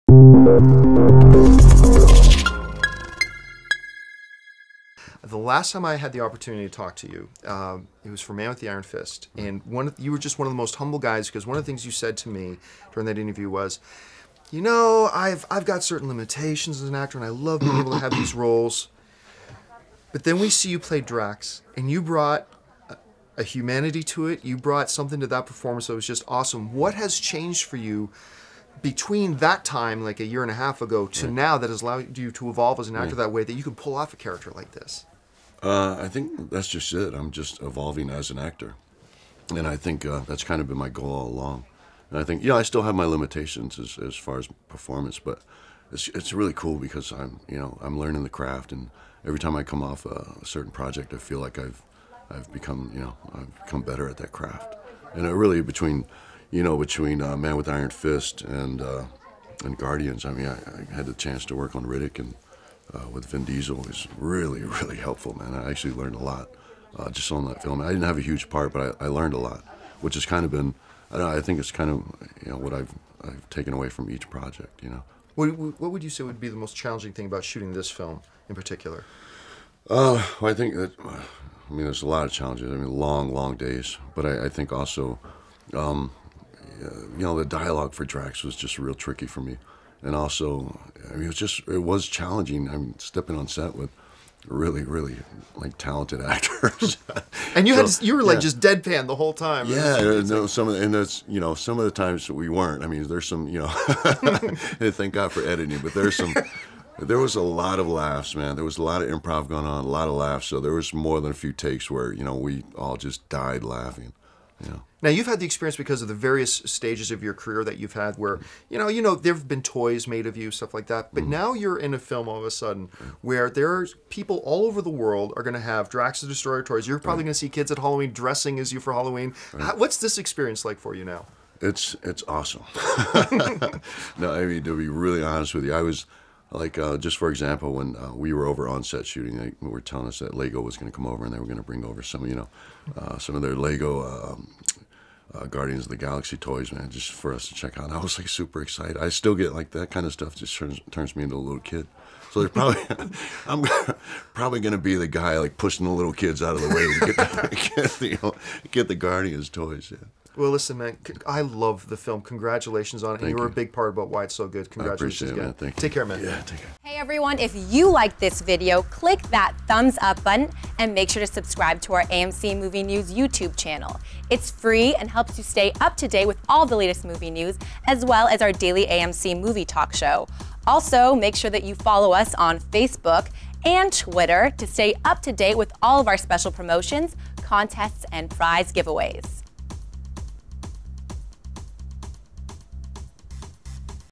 Dave Bautista Talks GUARDIANS OF THE GALAXY With AMC
Dave Bautista sat down with AMC's John Campea to talk about his upcoming film Guardians Of The Galaxy which opens up in AMC theatres everywhere on August 1, 2014.